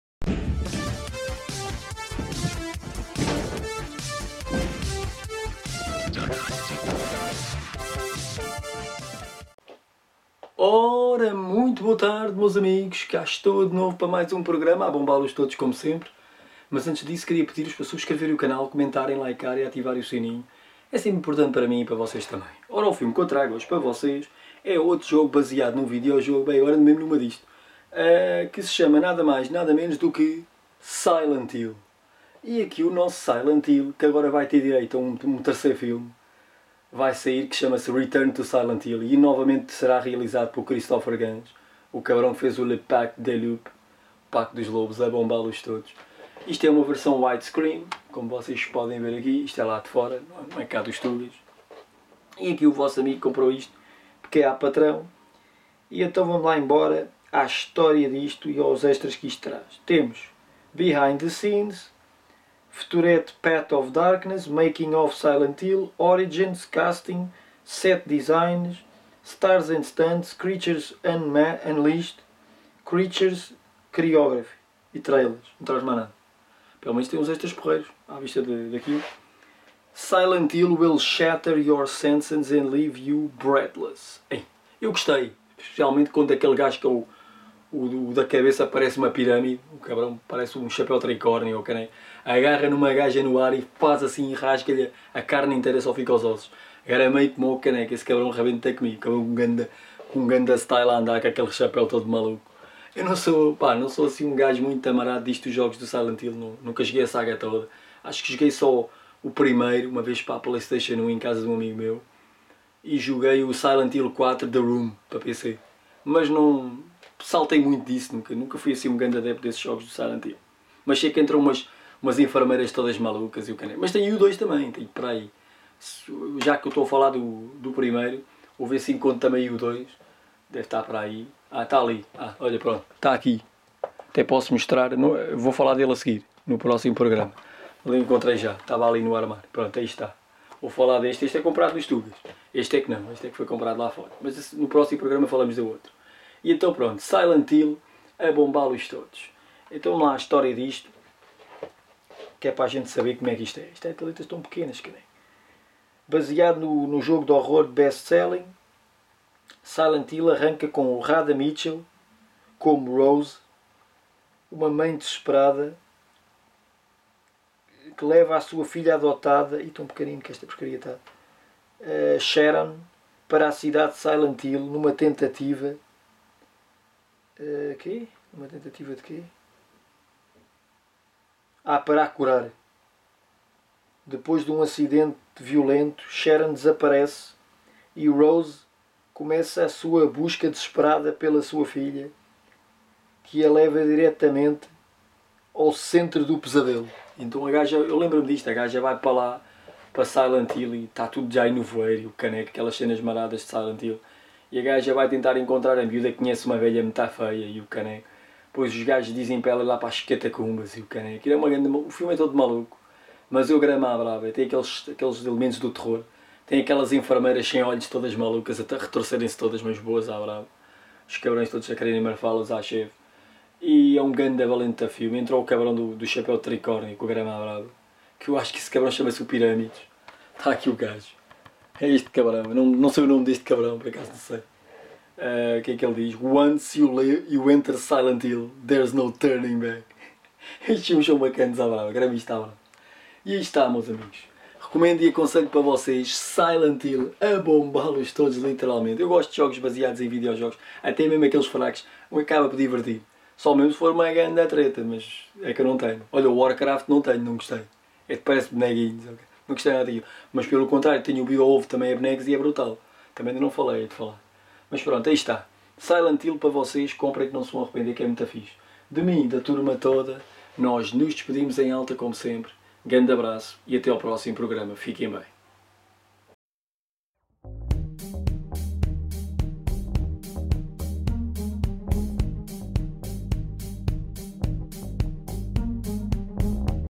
Review - 859 - Silent Hill - 2006
Plot : A woman, Rose, goes in search for her adopted daughter within the confines of a strange, desolate town called Silent Hill.